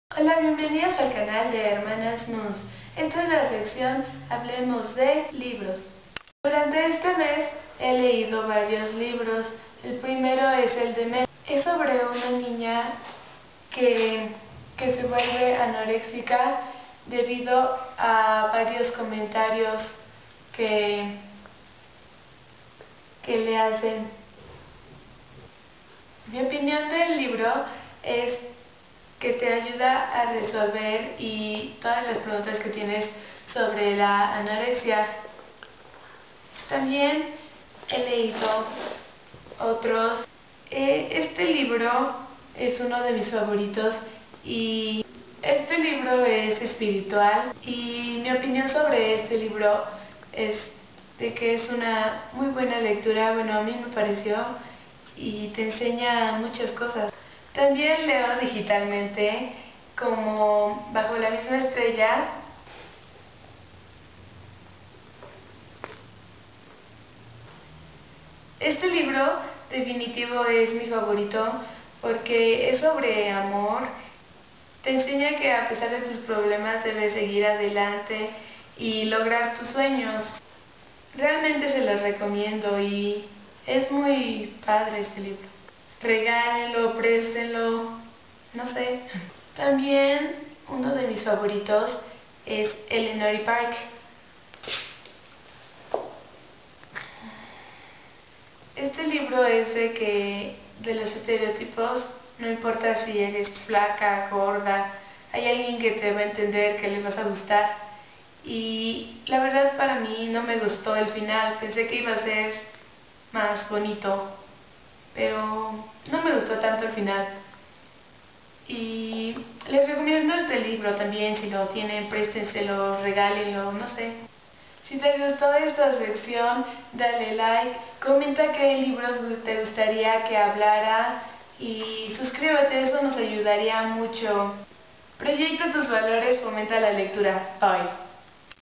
♡ Hablemos de: La cabaña/Melany/Bajo la misma estrella/Elenor y Park
Hola✌
Estoy son lo libros con sus autores

La cabaña - WM.Paul Young
Melany - Dorothy J. Harris
Bajo la misma estrella - John Green
Elenor y Park - Rainbow Rowell
Todos estos libros los encuentras en paginas de internet